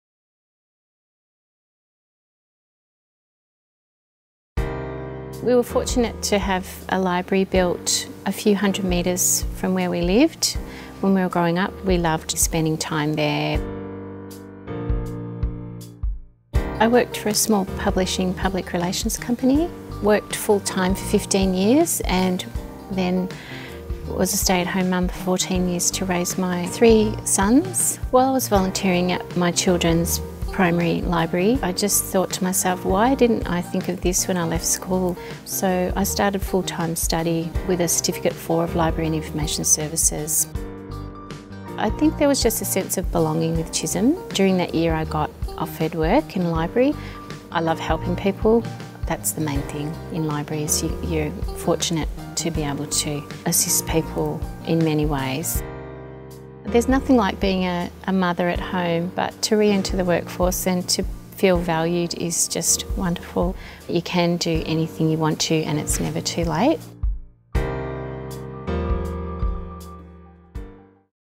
Diploma of Library and Information Services : Jill's Story
Find out how you can get qualified in Library and Information Services and inspire others.